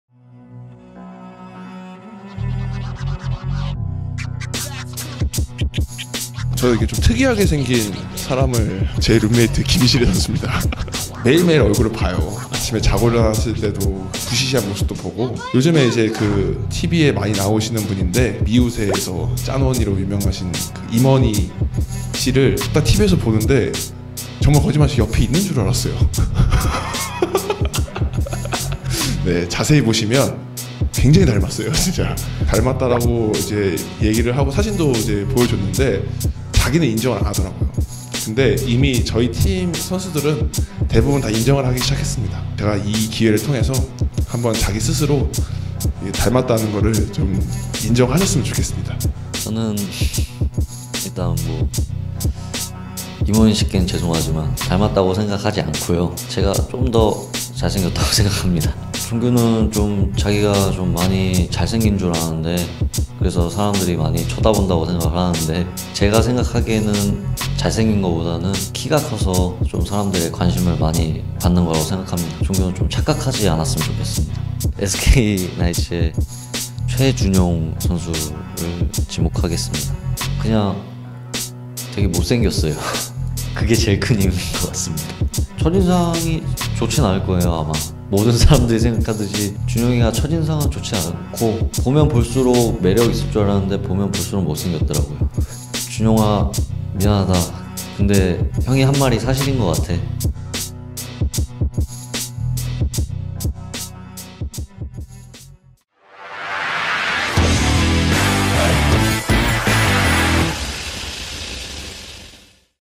김시래 "OOO, 보면 볼 수록 못생겨'
'SHOW ME THE KBL' 디스배틀
⑤ 창원 LG 김시래

김종규 디스를 들은
'짠원희' 김시래의 반격

김시래가 지목한 보면 볼 수록
못생긴(?) 다음 선수는?

지금 바로 영상으로 만나보시죠!